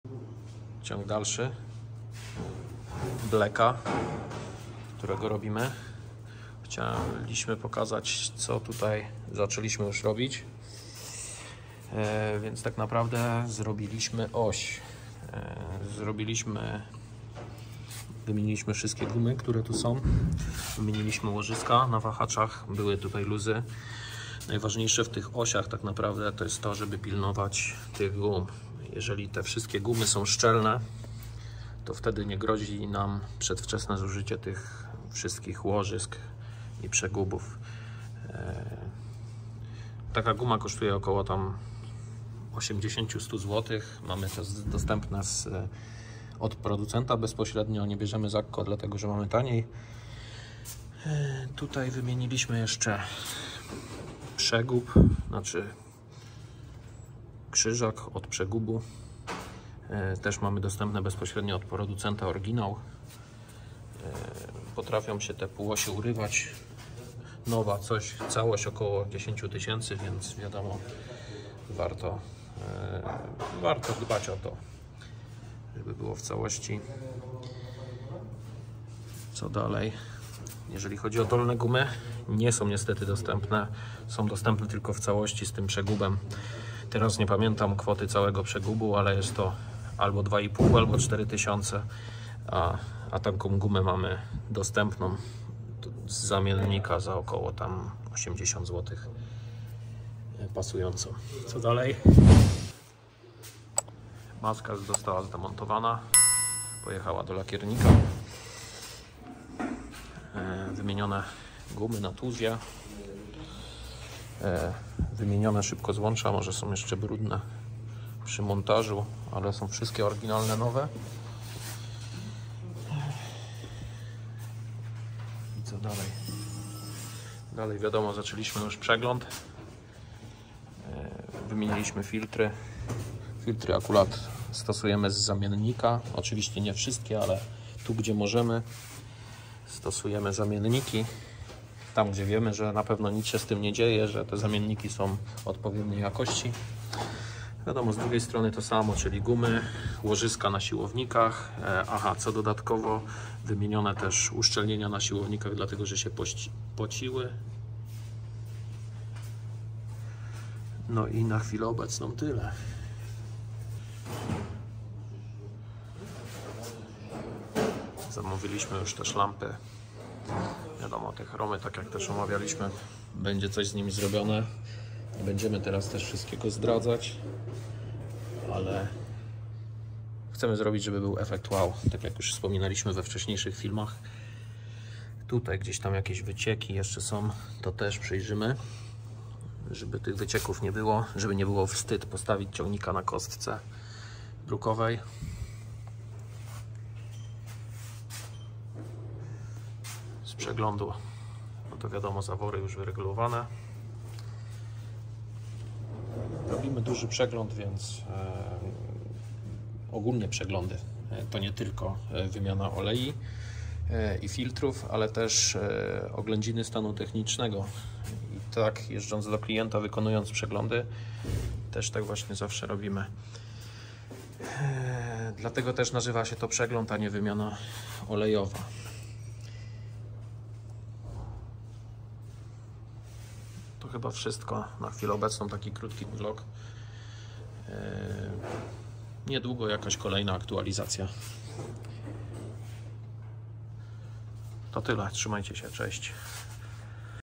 GRUBY PROJEKT cz.2 FENDT 927 VARIO BLACK
Ciąg dalszy przygotowania ciągnika do sprzedaży.
Na filmie pokazujemy czego pilnować, aby wydłużyć eksploatacje wahaczy przednich.

Serwis ciągników rolniczych Fendt, sprzedaż części.